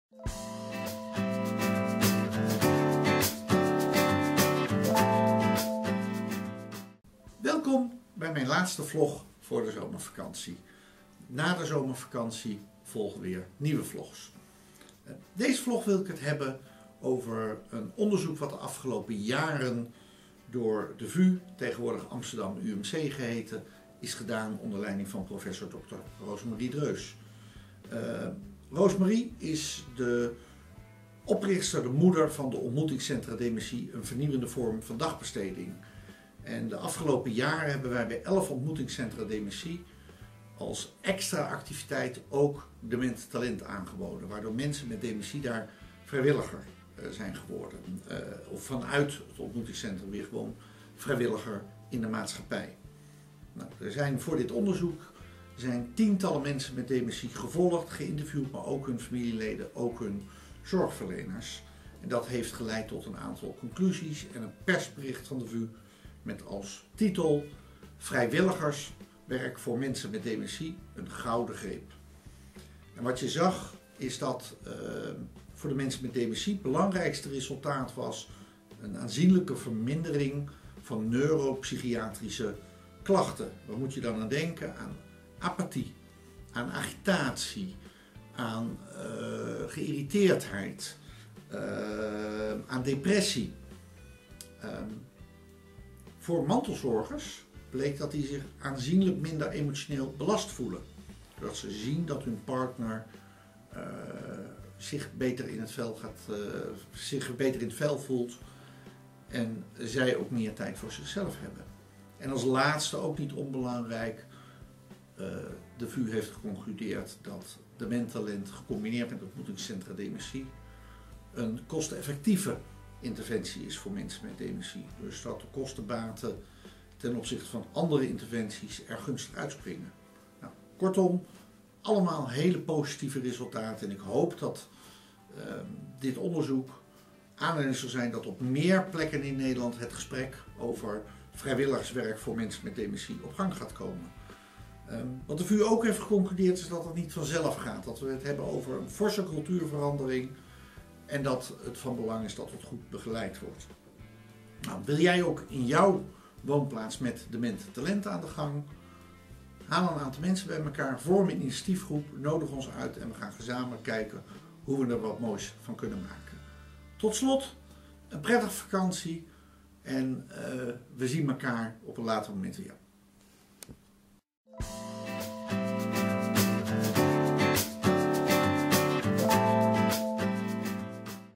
DementieVlog 58, Vrijwilligerswerk door mensen met dementie gouden greep
Afgelopen jaren werd wetenschappelijk onderzoek gedaan naar de toegevoegde waarde van DemenTalent. Deze vlog gaat over de resultaten van dit onderzoek. Hopelijk inspireert deze vlog vele mensen om in de eigen woonplaats te starten met DemenTalent.
Het Tv-programma Nieuwsuur besteedde op 16 juni 2019 aandacht aan dit onderzoek
Het persbericht over dit onderzoek is onder andere te lezen op
Deze DementieVlogs verschijnen tweewekelijks. Mijn DementieVlogs zijn een combinatie van voorlichting en een gesproken column. of mijn YouTube-kanaal.